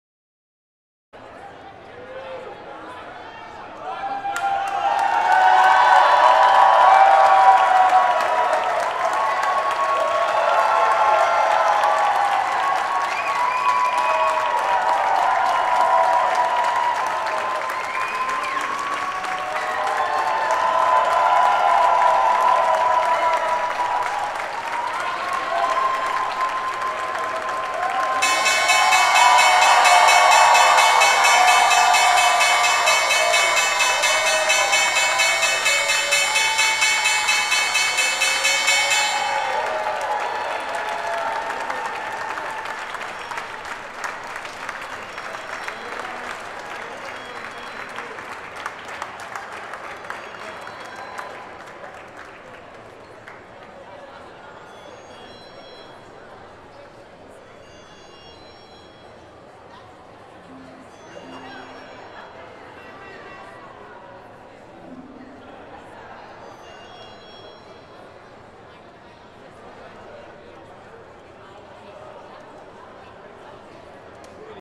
SAP SE (NYSE: SAP) Rings The Opening Bell®
The New York Stock Exchange welcomes SAP SE (NYSE: SAP) in celebration of its 25th anniversary of listing. To honor the occasion, Anthony Coletta, Chief Investor Relations Officer, will ring The Opening Bell®.